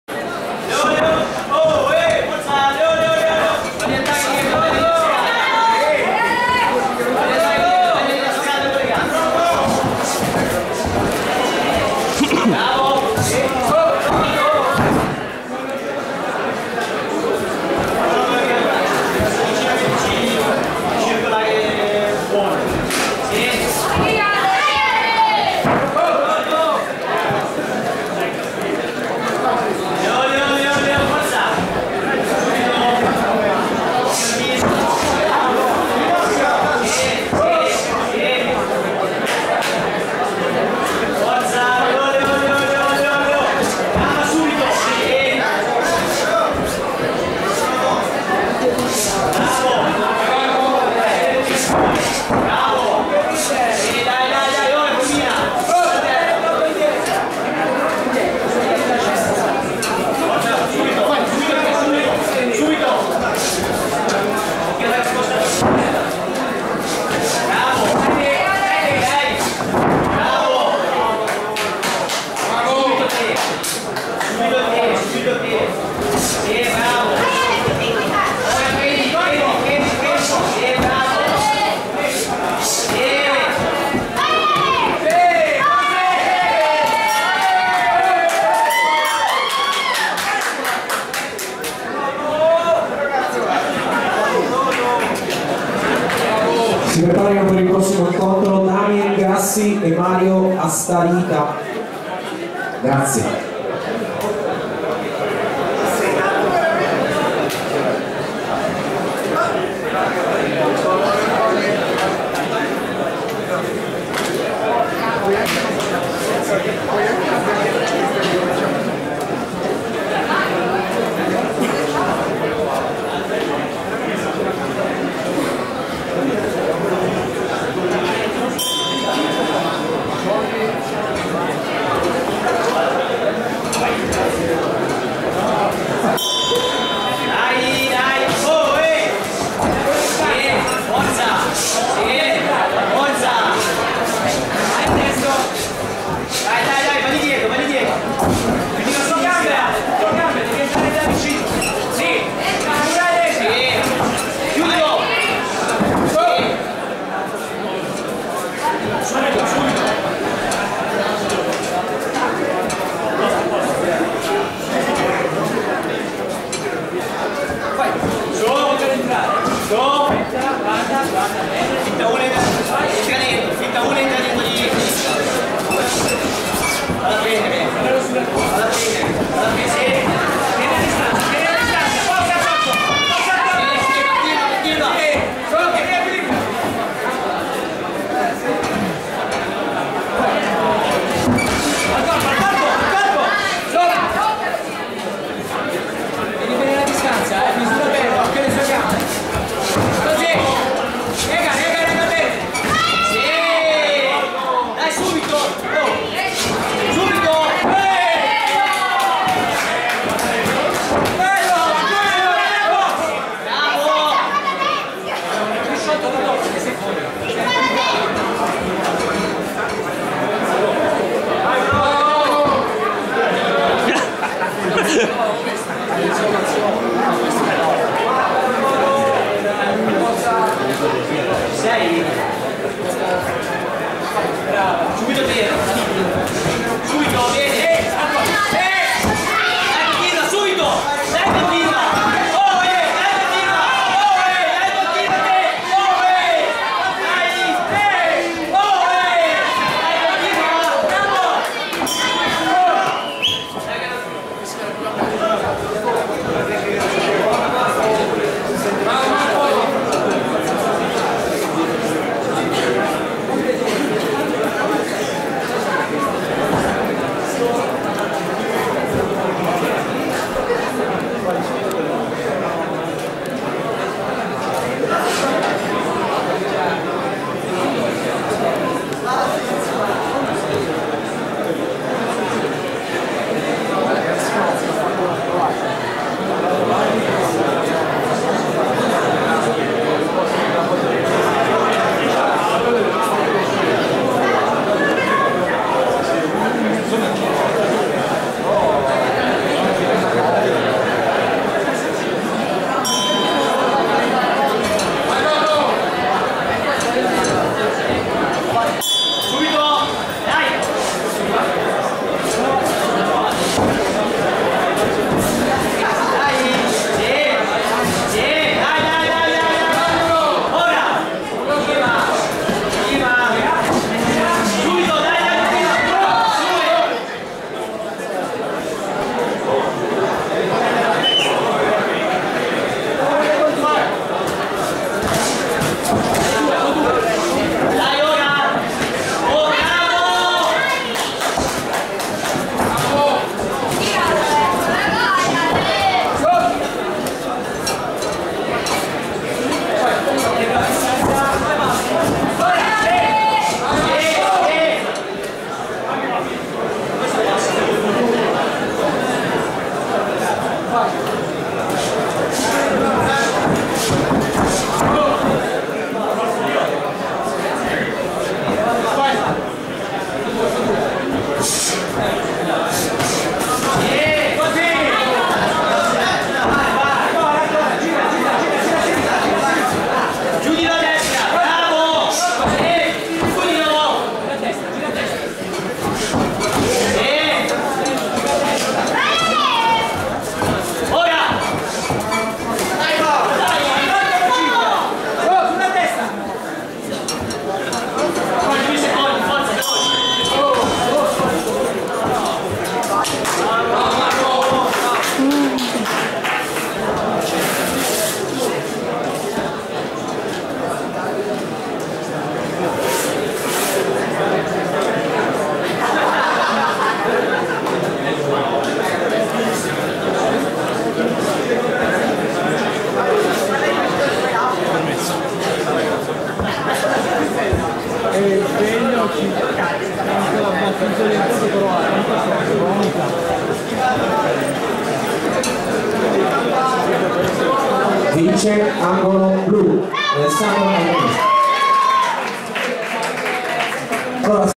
Reactive Club - Alessandro Albanese Fight Code Rules Light- 08/02/15 Scalenghe (TO)
Incontro Fight Code Rules Light degli atleti della palestra Reactive Club di Torino presso Scalenghe (prov. Torino - Turin).